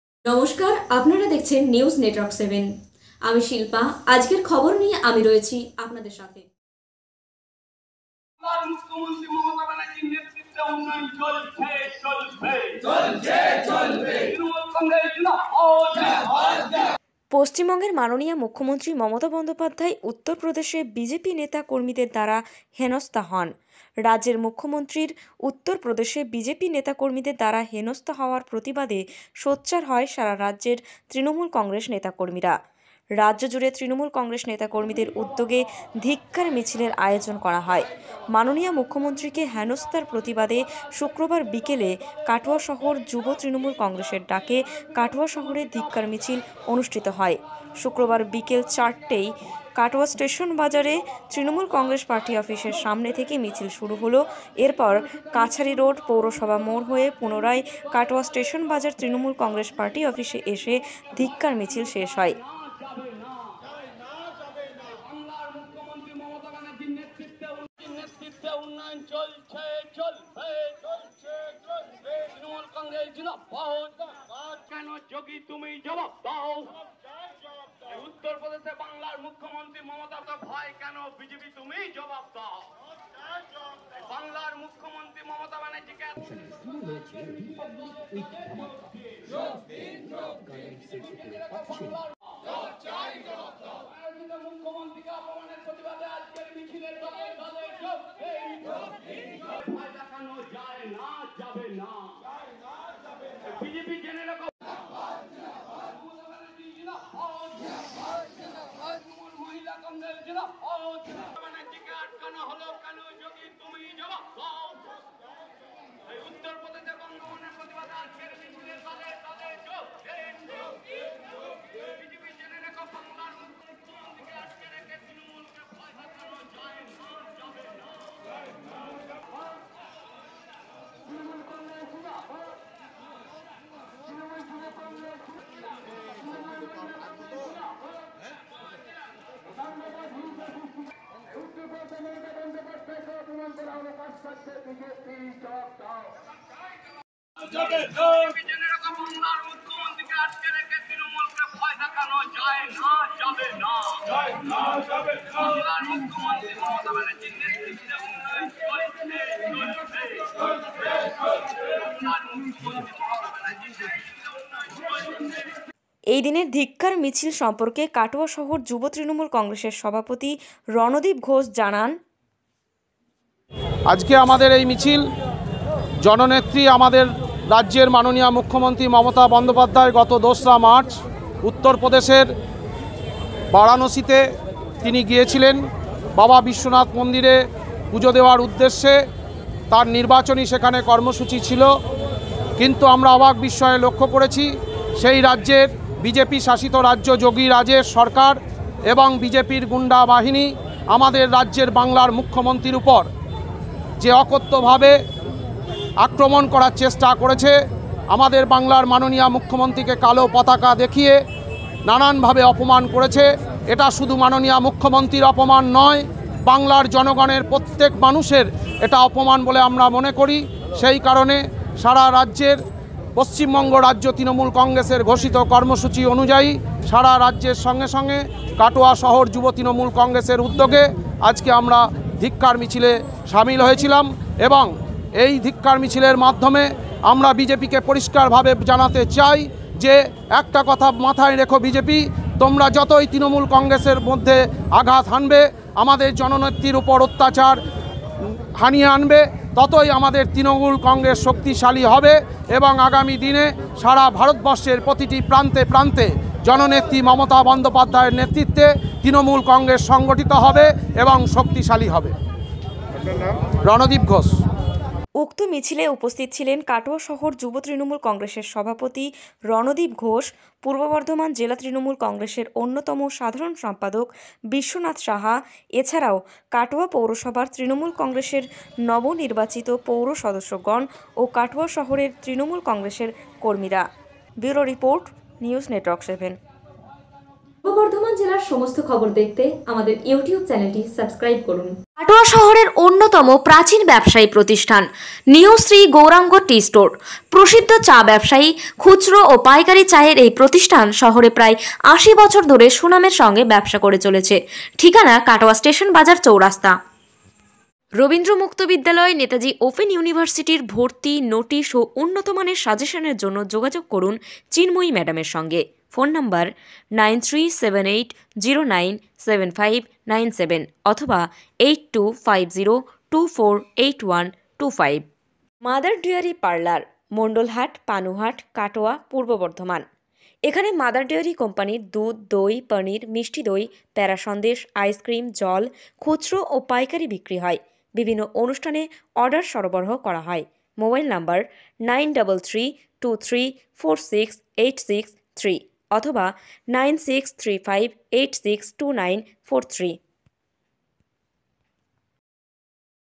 Katwa Shohor Jubo TMC -er Dake Dhikkar Michhil 05/03/2022
কাটোয়া শহর যুব তৃণমূল কংগ্রেসের ডাকে ধিক্কার মিছিল 05/03/2022